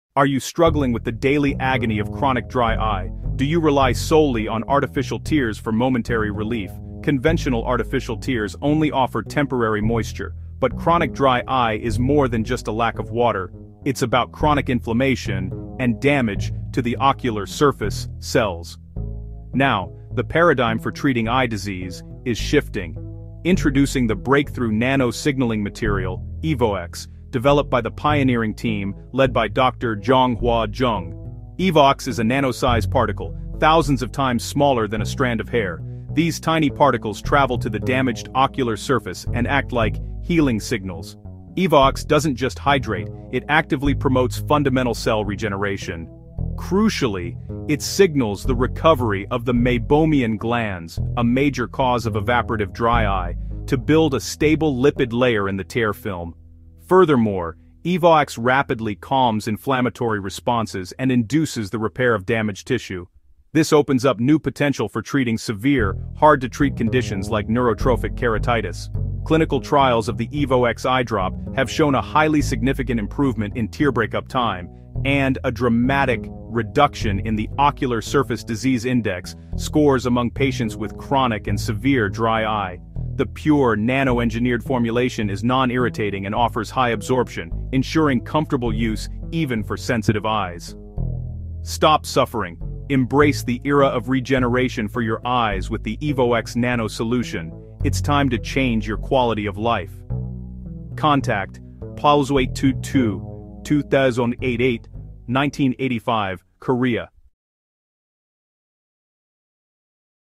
From Dryness to Clarity EvoX Nano Signals Heal
Are you struggling with the daily agony of chronic dry eye? Do you rely solely on artificial tears for momentary relief? Conventional artificial tears only offer temporary moisture. But chronic dry eye is more than just a lack of water— s about chronic inflammation and damage to the ocular surface cells. Now, the paradigm for treating eye disease is shifting.